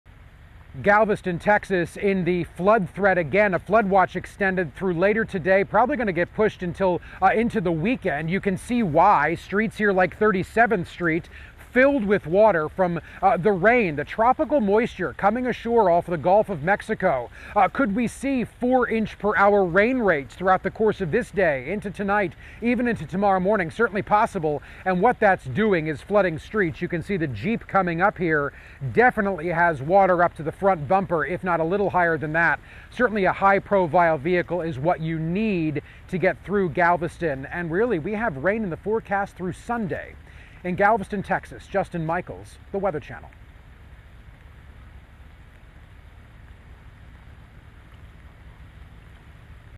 Flash Flooding in Galveston, TX
The Weather Channel's Justin Michaels reports from Galveston, Texas where flash flooding has hit the area.

Credit: The Weather Channel // Date: 7/27/24